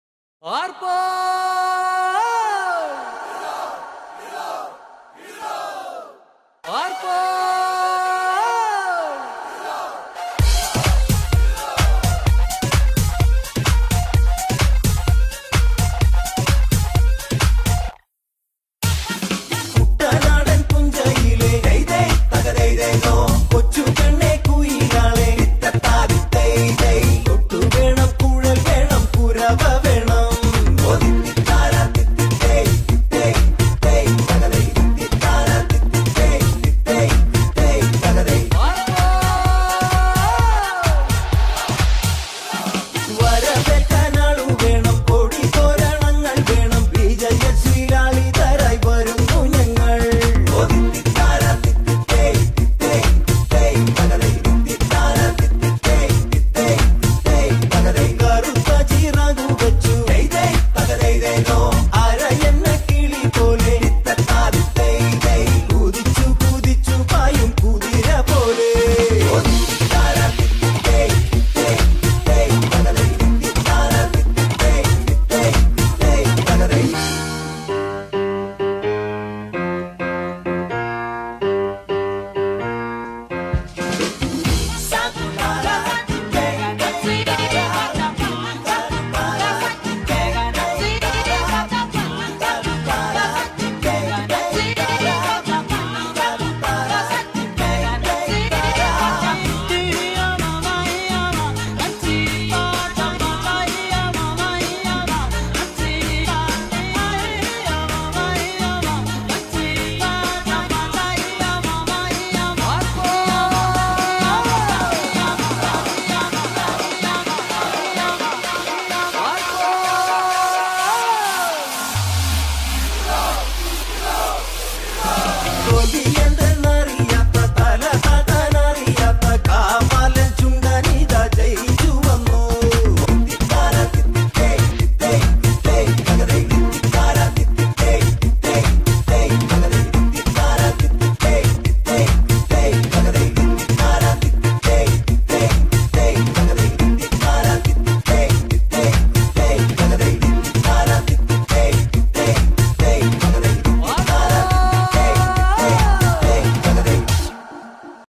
Rafting in the Ganges, Rishikesh - Go Pro Hero 3+
Rafting in the upper reaches of Ganges in Rishikesh. The water is so pure up here that the ganges actually feels holy and divine! And the 26 KM rafting was a moderate one. Superb rapids all along and an amazing team to add to the fun!
Edited by a friend, Sreejith. Song is a remix of the traditional Kerala boat song! Entire video shot on GoPro Hero 3+ black with a head and wrist mount.